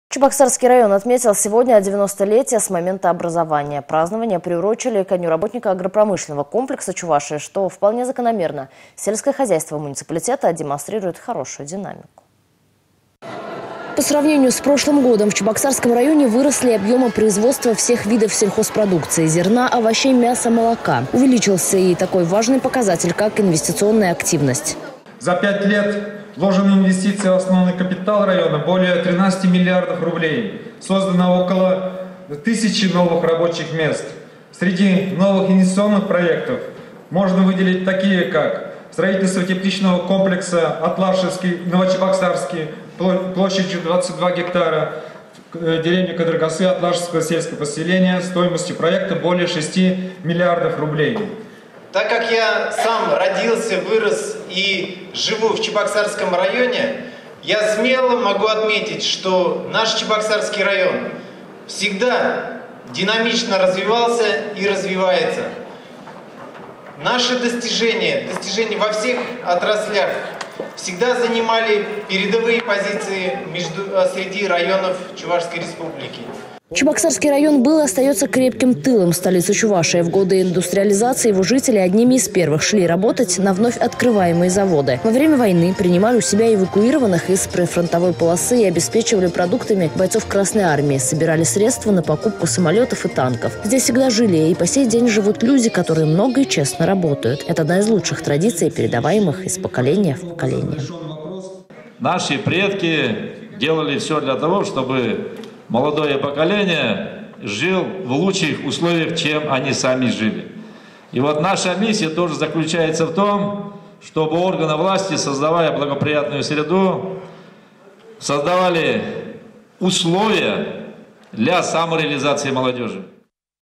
Чебоксарский район отметил 90-летие с момента образования.
Празднование приурочили к Дню работника агропромышленного комплекса Чувашии, что вполне закономерно — сельское хозяйство муниципалитета демонстрирует хорошую динамику.